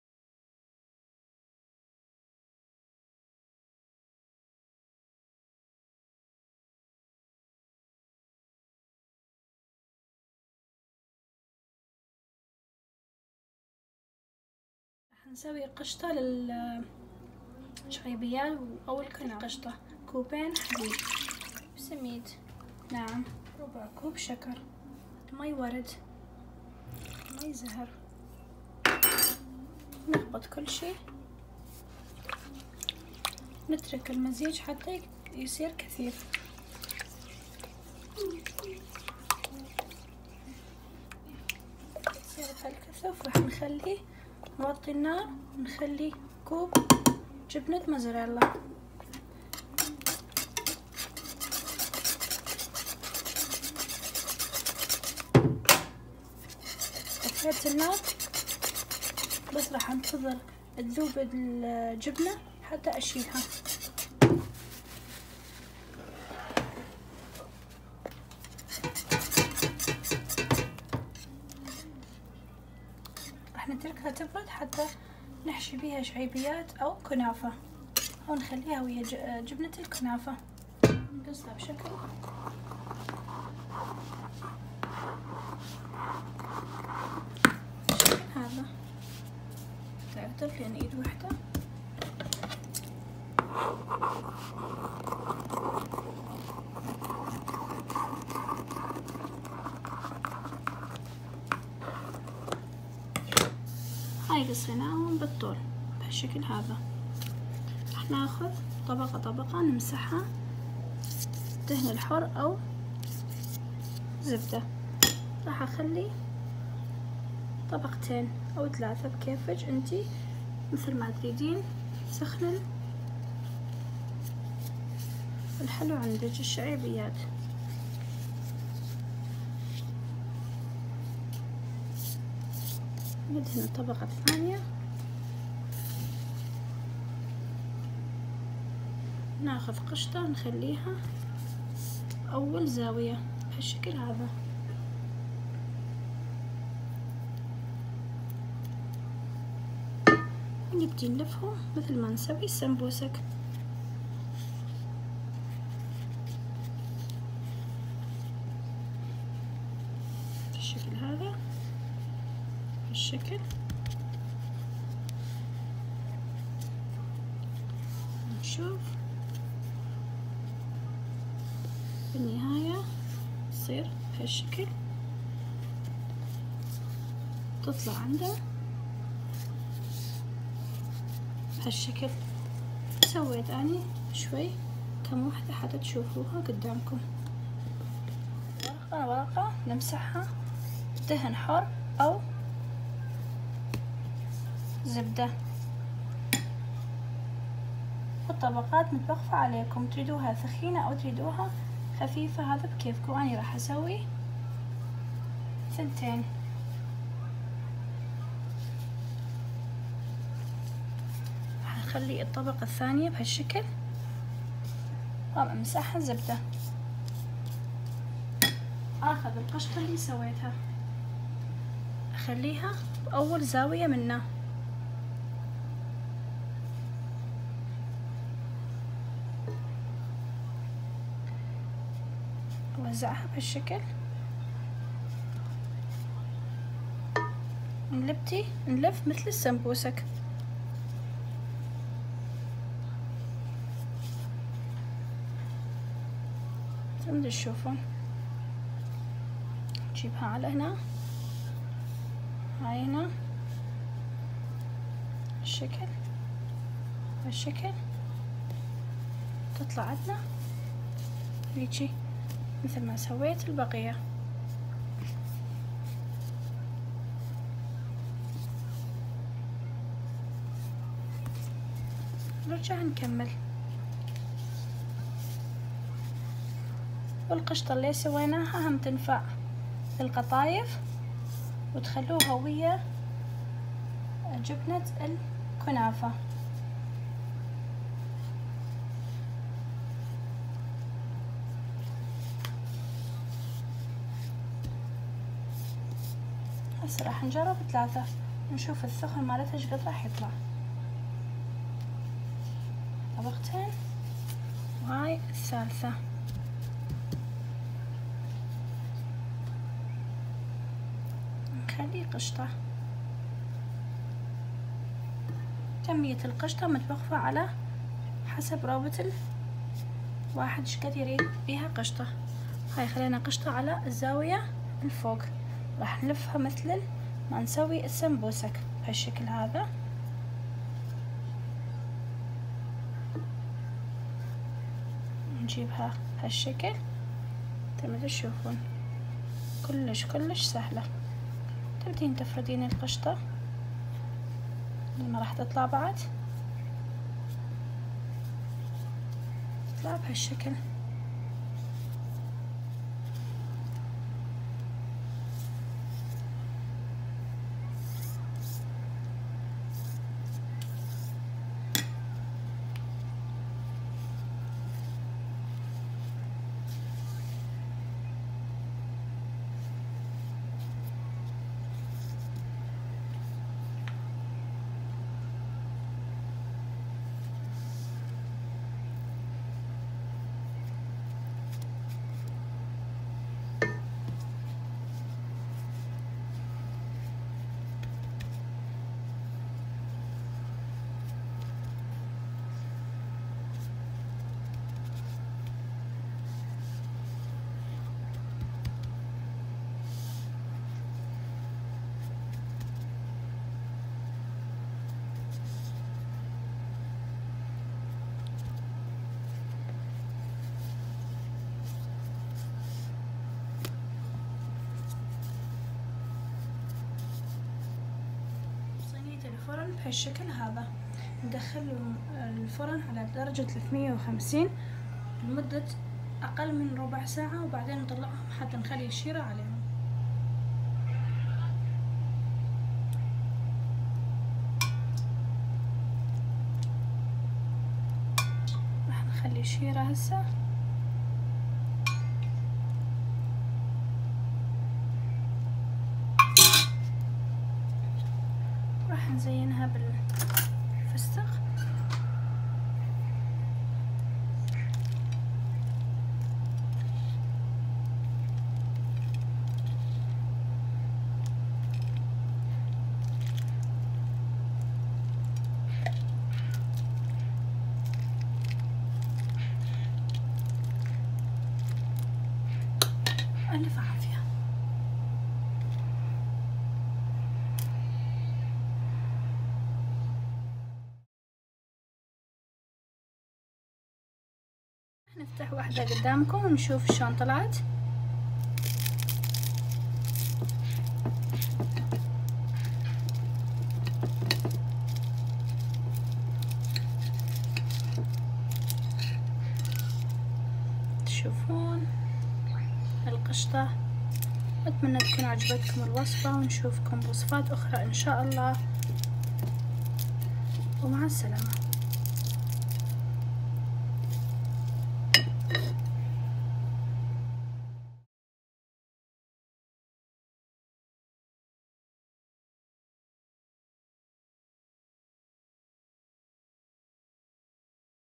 شعيبيات بالقشطة
القشطة
كوبين حليب كامل الدسم
كوب سميد
ربع كوب شكر
علبتين قشطة
ملعقة مي ورد
ملعقة مي زهر
كوب جبنة موزريلا